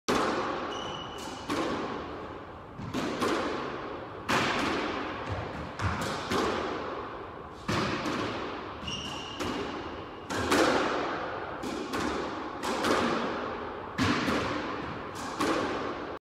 Always Practice Your Nicks
If you’re going to go for nicks in your matches, practice them...all..of..the..time!